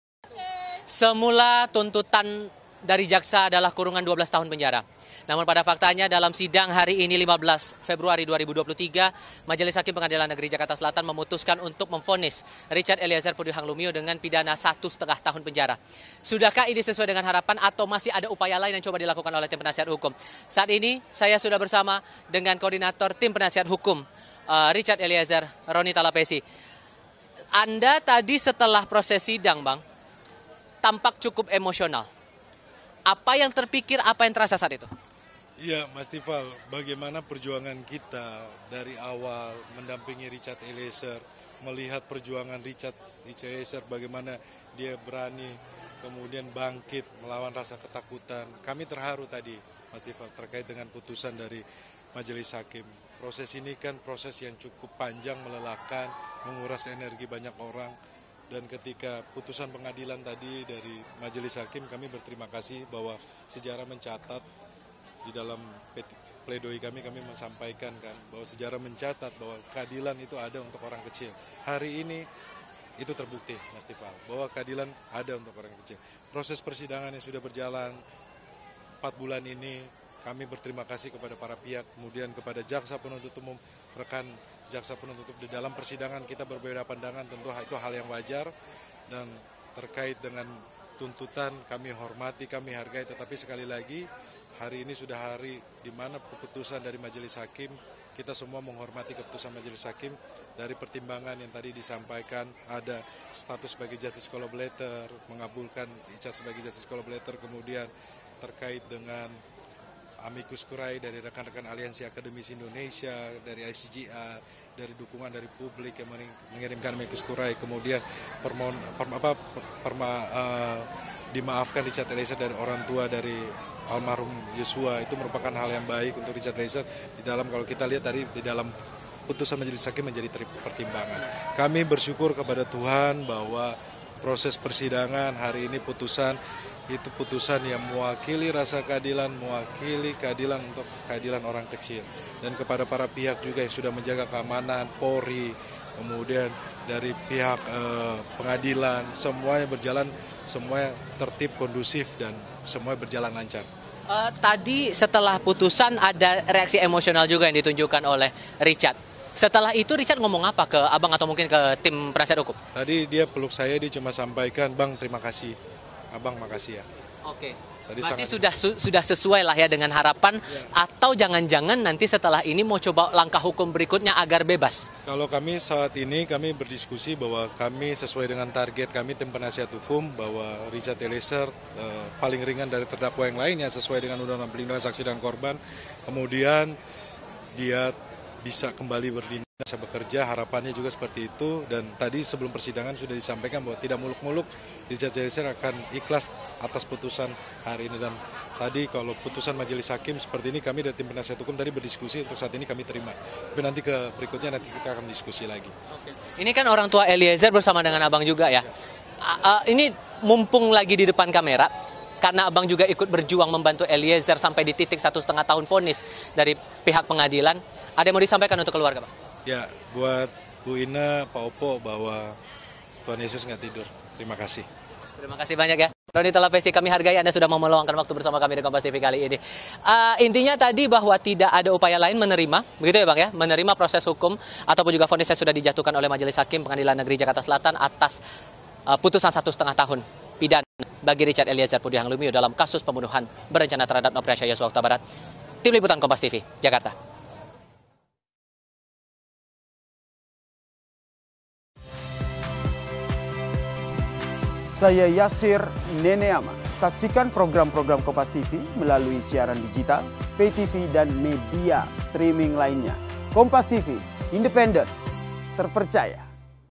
Terungkap yang Disampaikan Richard Eliezer pada Pengacara Usai Divonis 1,5 Tahun di Kasus Sambo
JAKARTA, KOMPASTV - Richard Eliezer divonis 1,5 tahun penjara atau 1 tahun 6 bulan penjara dalam kasus pembunuhan Brigadir Yosua.

Hal tersebut disampaikan hakim pada sidang putusan, Rabu (15/2) di PN Jakarta Selatan.

Putusan tersebut disambut baik dan haru oleh pengacara Richard Eliezer, Ronny Talepessy.

Baca Juga Terharu! Momen Tangis Richard Eliezer Pecah saat Hakim Jatuhi Vonis 1 Tahun 6 Bulan Penjara di

"Bagaimana perjuangan kita dari awal mendampingi Richard, melihat perjuangan Richard Eliezer, berani, bangkit, merasa ketakutan, kami terharu tadi," ucap Ronny pada KompasTV.

"Sejarah mencatat keadilan itu ada untuk orang kecil, hari ini itu terbukti," lanjutnya.

Ronny mengungkap hal yang disampaikan Richard Eliezer usai divonis ringan oleh hakim.

"Tadi saya peluk saya dan sampaikan 'Bang, terima kasih," ucap Ronny.

Richard Eliezer divonis 1,5 tahun penjara atau 1 tahun 6 bulan penjara dalam kasus pembunuhan Brigadir Yosua.